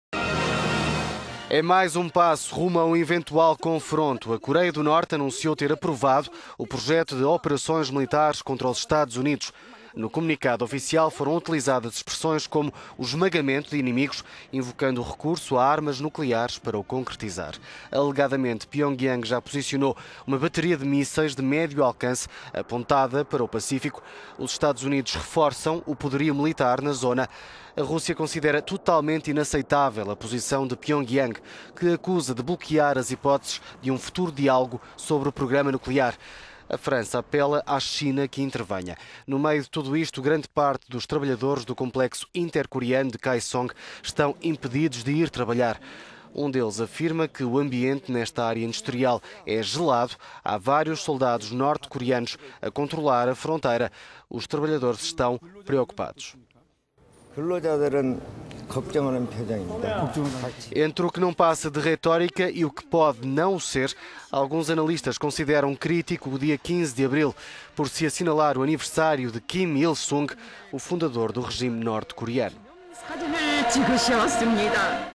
Coreia do Norte já definiu planos militares contra EUA
É mais passo rumo a um eventual confronto: a Coreia do Norte anunciou ter aprovado o projeto de operações militares contra os Estados Unidos. No comunicado oficial foram utilizadas expressões como o "esmagamento" de inimigos, invocando o recurso a armas nucleares para o concretizar.

Alegadamente, Pyongyang já posicionou uma bateria de mísseis de médio alcance apontada para o Pacífico.

Os Estados Unidos reforçam o poderio militar na zona. A Rússia considera "totalmente inaceitável" a posição de Pyongyang, que acusa de bloquear as hipóteses de um futuro diálogo sobre o programa nuclear. A França apela à China que intervenha.

No meio de tudo isto, grande parte dos trabalhadores do complexo intercoreano de Kaesong estão impedidos de ir trabalhar. Um deles afirma que "o ambiente nesta área industrial é gelado. Há vários soldados norte-coreanos a controlar a fronteira. Os trabalhadores estão preocupados."

Entre o que não passa de retórica e o que pode não o ser, alguns analistas consideram crítico o dia 15 de abril, por se assinalar o aniversário de Kim Il-Sung, o fundador do regime norte-coreano.